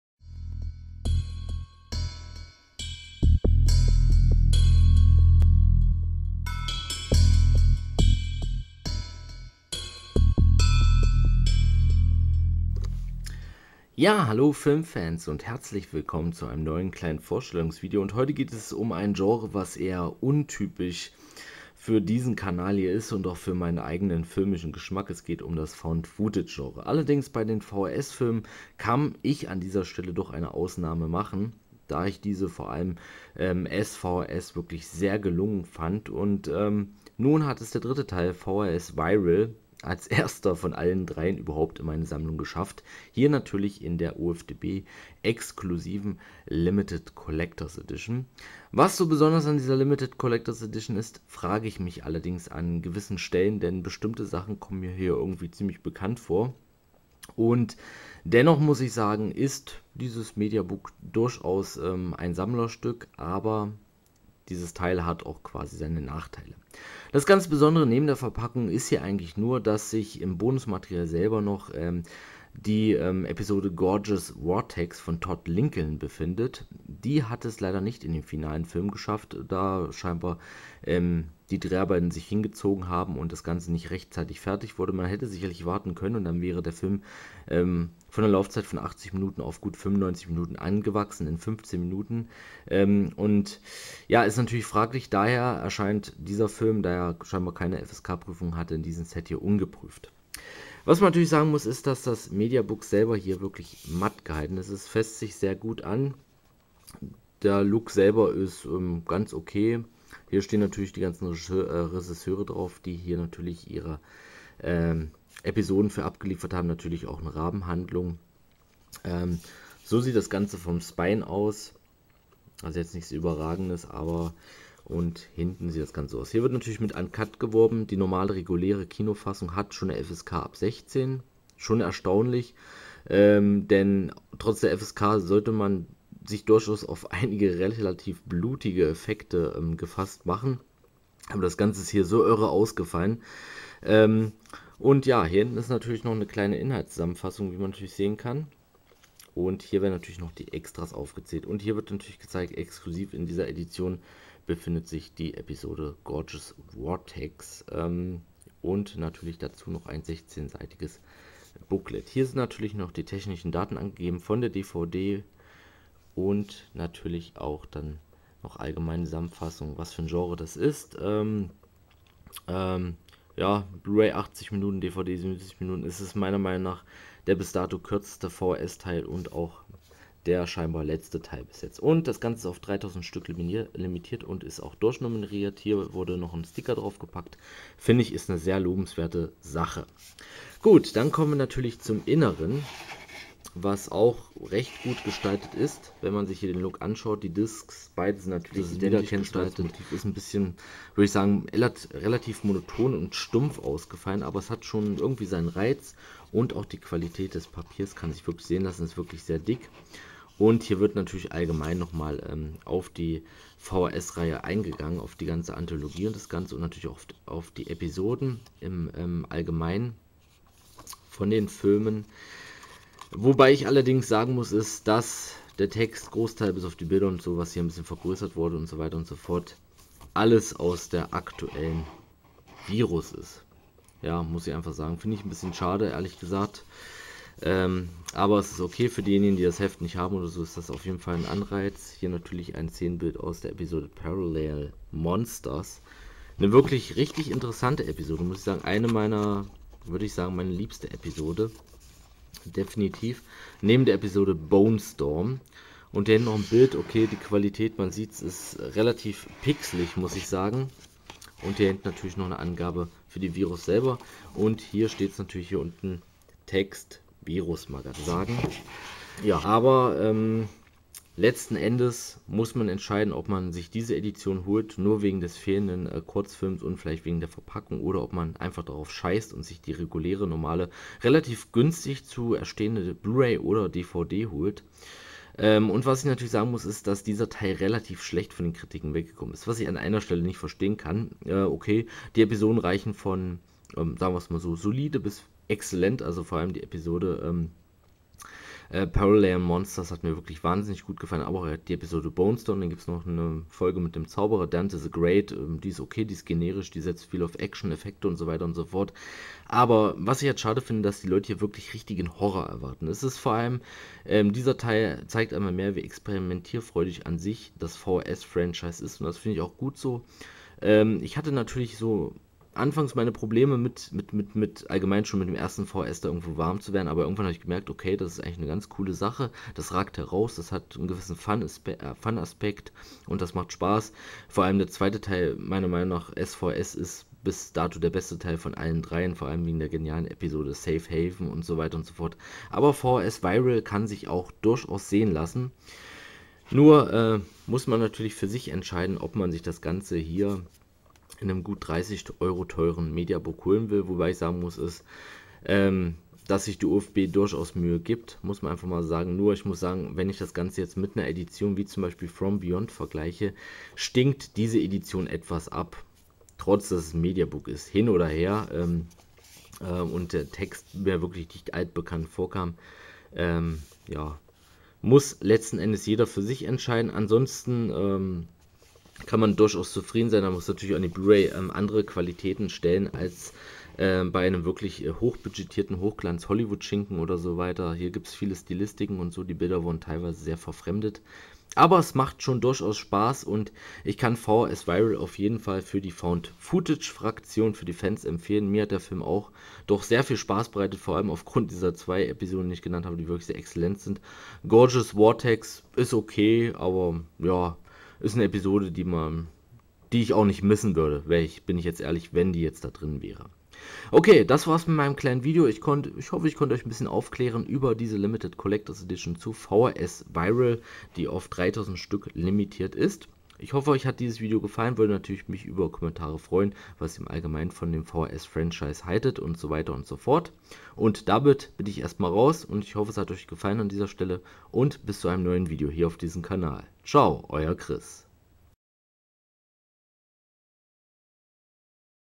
V/H/S - Viral - 2 Disc Limited Collector´s Edition -
Auf viralen Wege findet das Franchise rund um obskure Wackelfilmchen ihr Ende und das erneut nach "S - V/H/S" im Mediabook - Look mit ein paar Extra - Sperenzchen und wenigen sinnvollen Goodies.